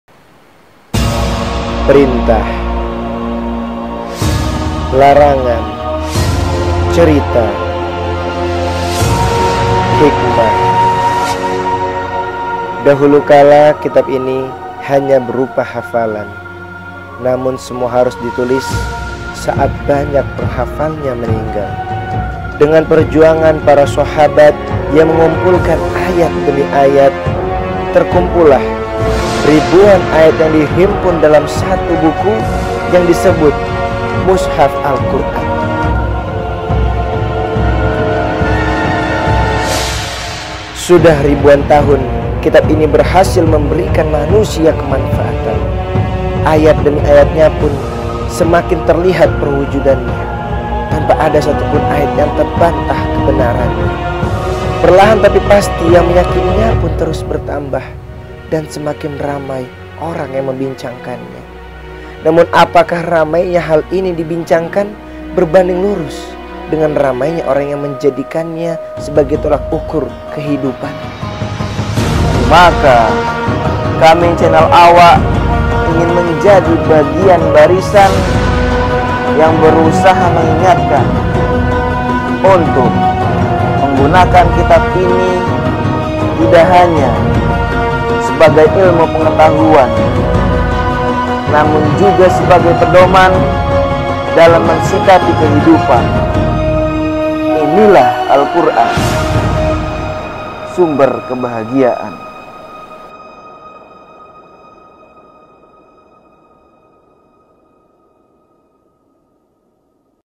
Official Trailer | Al-Qur'an The Way Of Life | CenelAwak
Memang sih uang membuat kita dapat membeli apaun yang kita mau.
Cuman bukan berarti untuk bahagia harus punya uang pren

Salah satu wujud adilnya allah adalah kebahagian adalah hak semua orang..yap semua orang yang mau mengikuti apa yang menjadi petunjukNya.

Allah berfirman dalam suroh albaqoroh ayat38
Bahwa Kalau kalian mau mengikuti apa yang menjadi petunjuku
nggak ada kekhawatiran dan nggak ada kesusahan

Jadi Kunci dari bahagia itu simple banget
nggak butuh duit banyak
nggak butuh rumah mewah
bahkan nggak perlu istri cantik prenn..hehe
cukup ikuti aja qura'an

Maka kali ini dan insallah kedepanya cenelawak bakal bagi2 video yang insallah bermanfaat.Tentang cara mensikapi berbagai persoalan dunia dengan alquran..
cocok kln rasa??cocoklaa

Makanya Ikutinlah Truss..hehe